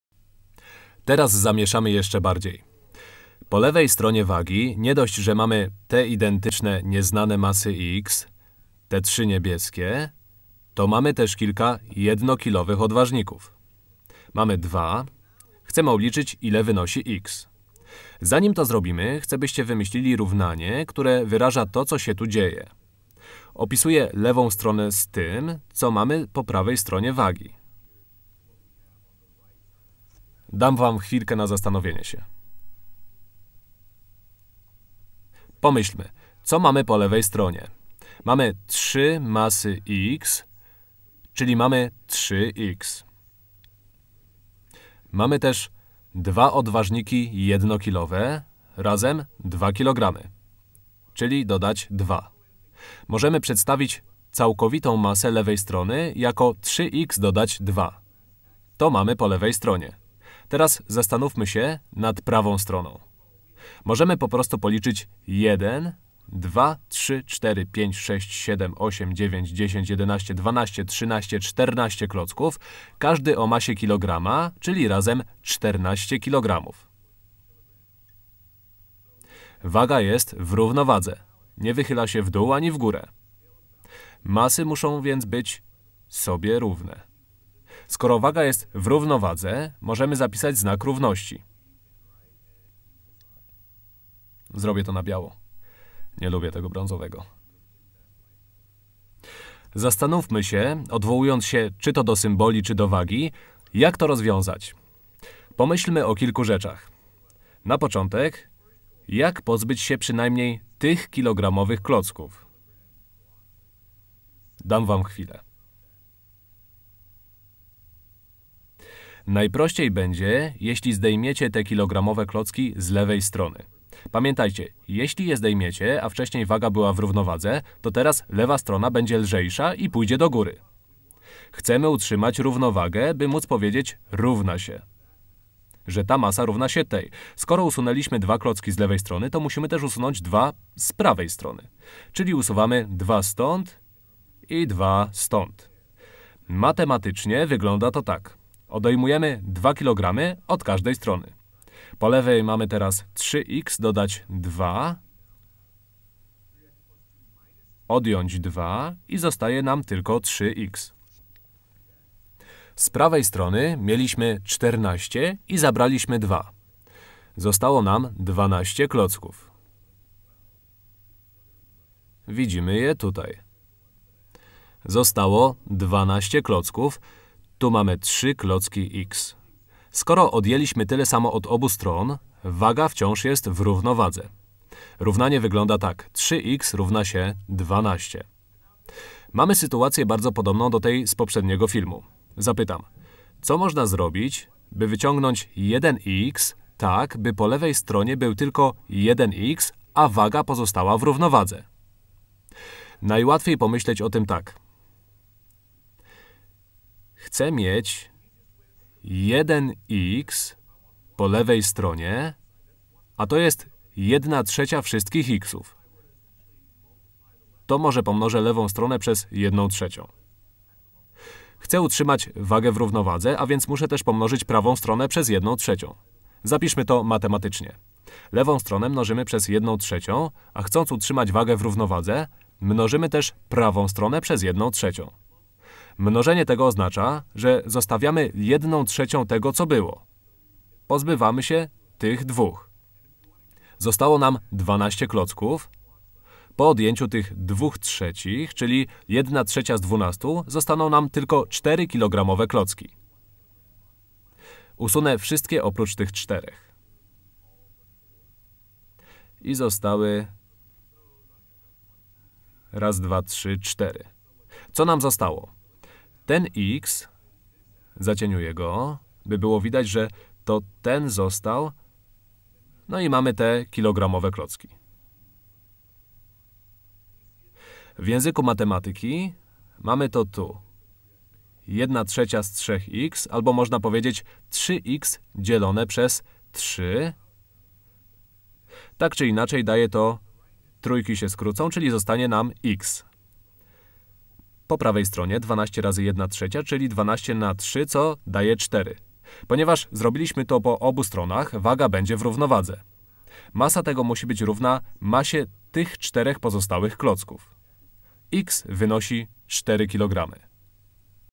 Dlaczego jeśli robimy coś po jednej stronie równania to musimy to samo zrobić po drugiej stronie
Ścieżka dźwiękowa wykonana na zlecenie Ośrodka Rozwoju Edukacji -"Scholaris - portal wiedzy dla nauczycieli" współfinansowana przez Unię Europejską w ramach Europejskiego Funduszu Społecznego. Czyta Dariusz Wieteska